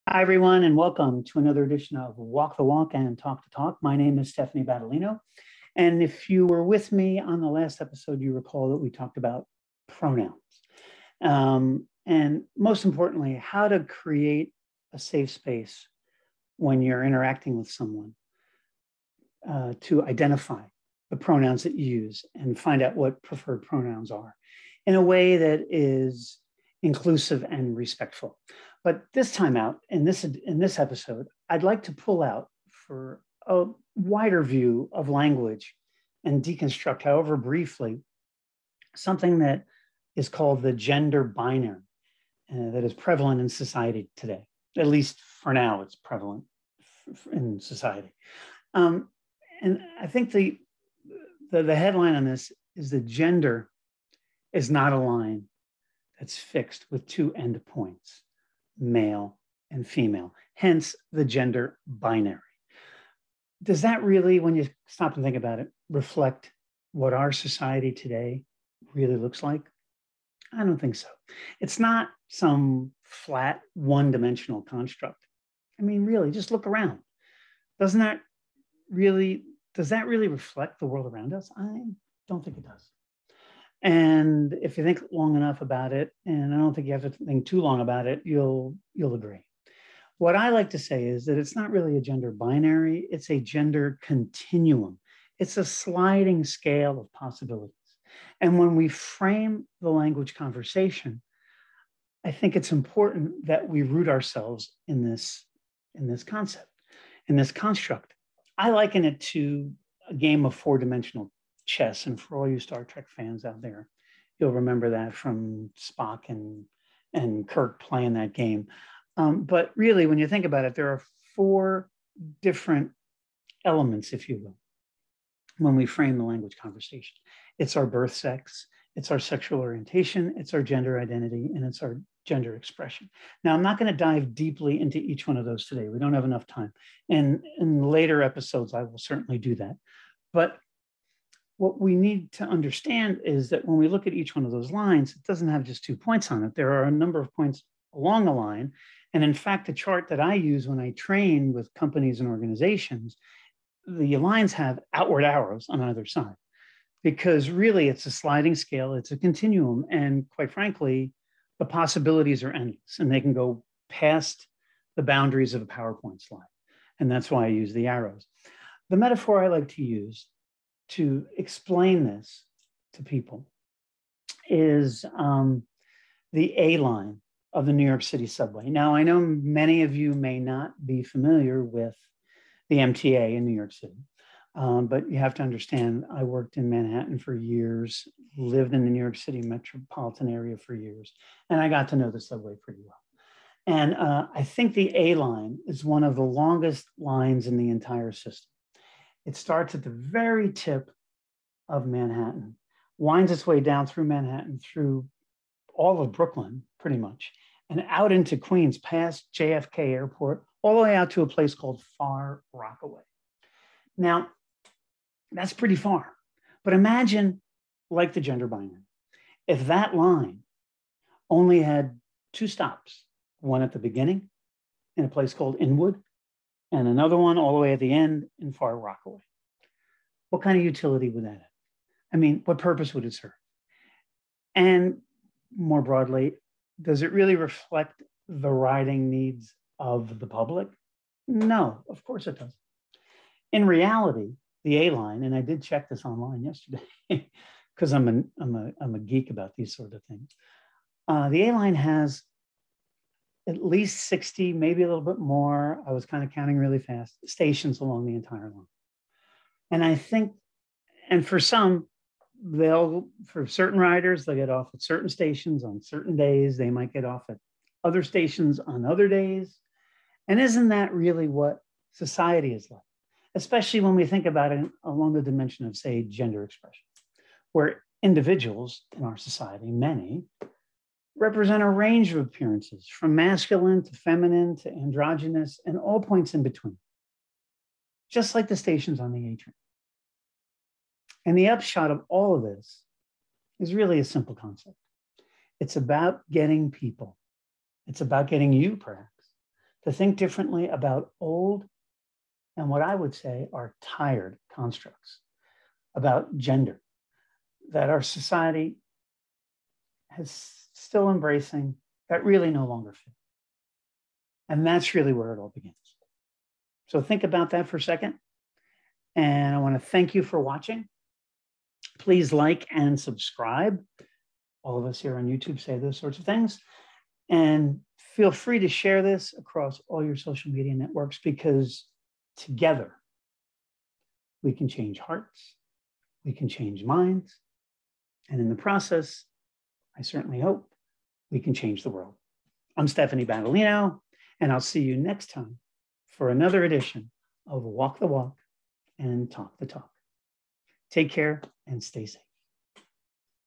The Gender Continuum
Let's talk about TheGenderBinary.

I believe gender is a sliding scale of possibilities. I like to call it The Gender Continuum.

When we frame the conversation around gender, I like to think of gender as having four elements.